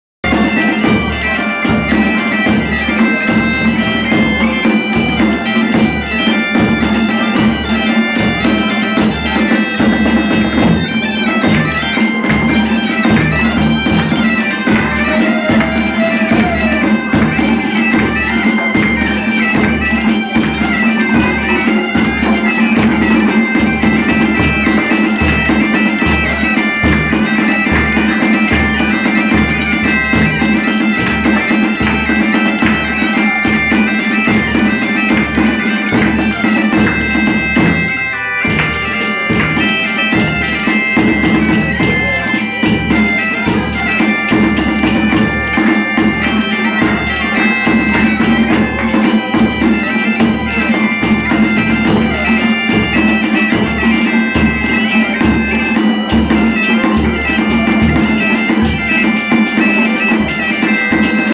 furancheiros
moncho e marcos tocando unha peza no furancho da balsa.